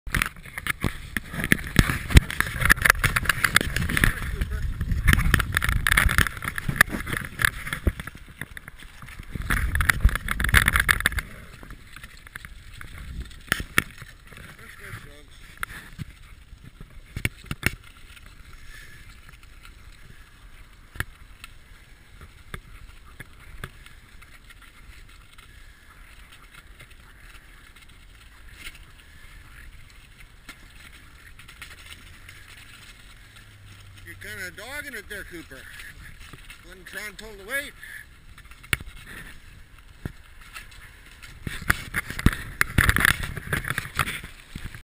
Picking up the Pie
Cooper and Tron- Sled-dogs in training... pressed into service delivering hot pie in Winnipeg.